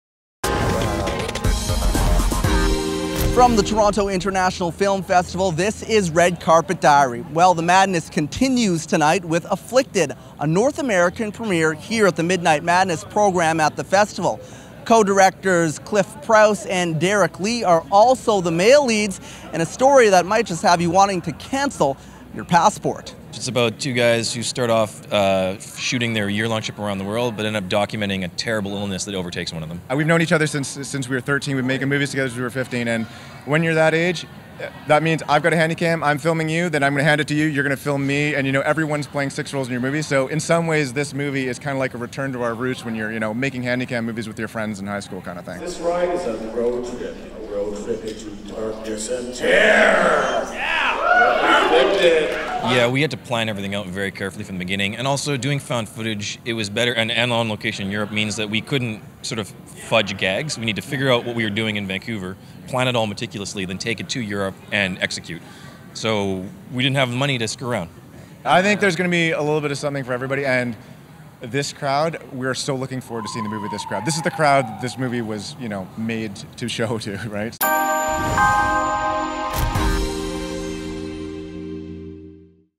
Red Carpet for "Afflicted" the horror film from directors Clif Prowse and Derek Lee
This feature debut follows two friends (played by co-directors Derek Lee and Clif Prowse) who set out to travel the world. But the trip soon takes a dark turn when one of the men shows signs of an affliction that gradually takes over his entire body. Distributed by OneLoad.com